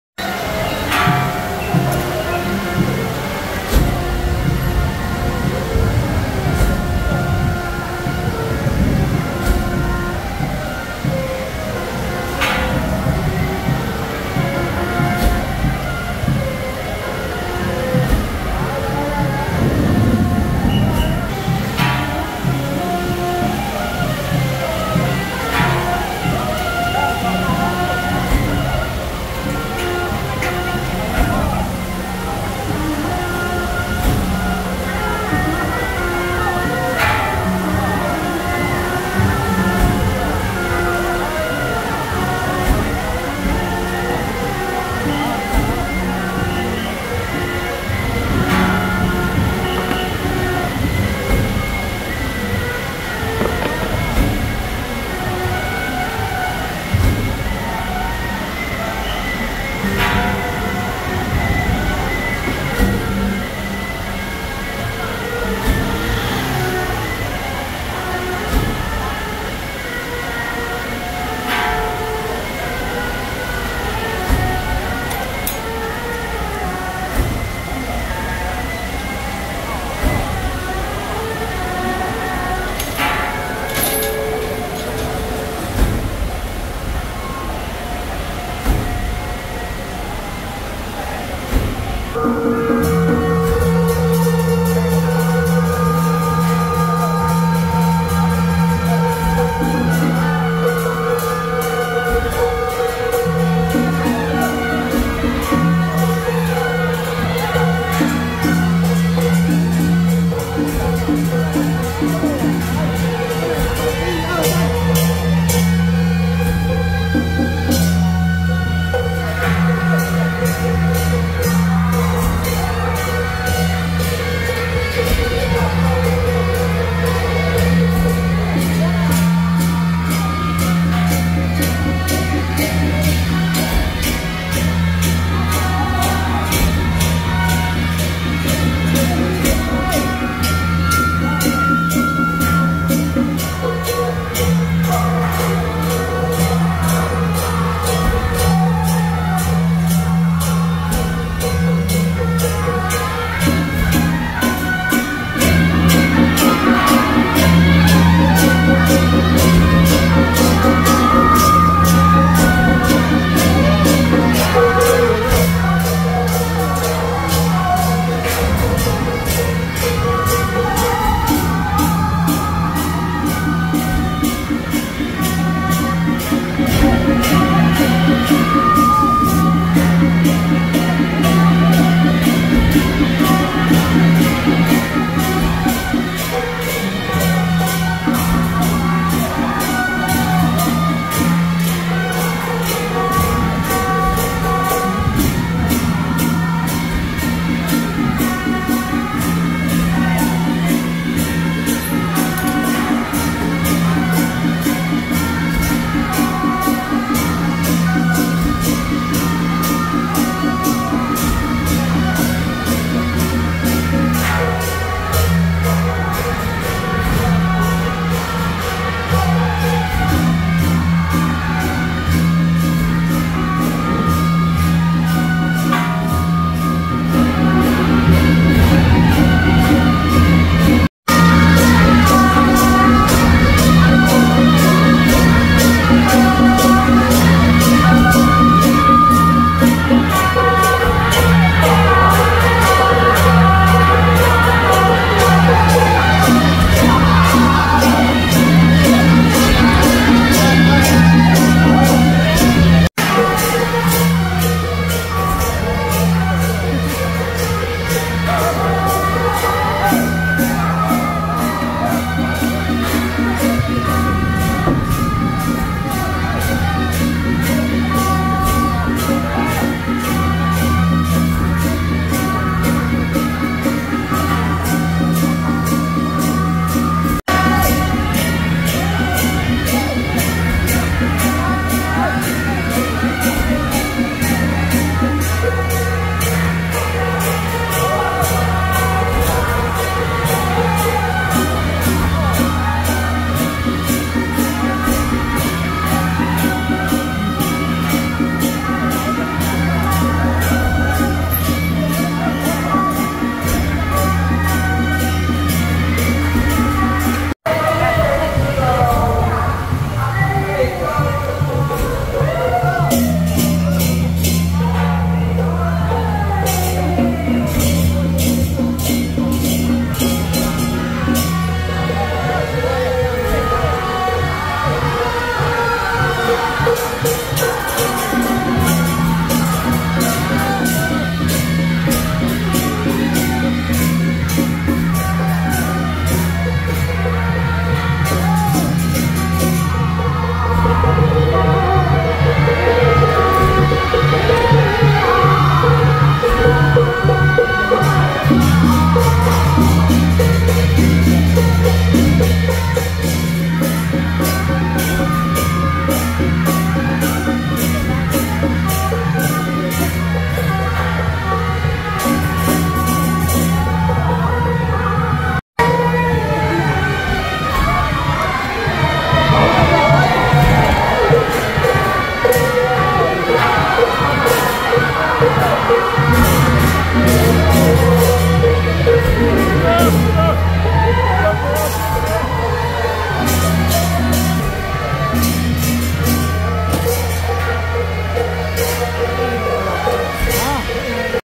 【關渡二媽＆石牌福星宮-關聖帝君,列位尊神 聖駕】~112石牌福星宮恭祝關聖帝君聖誕暨建宮270週年遶境
關渡二媽＆石牌福星宮-關聖帝君,列位尊神 聖駕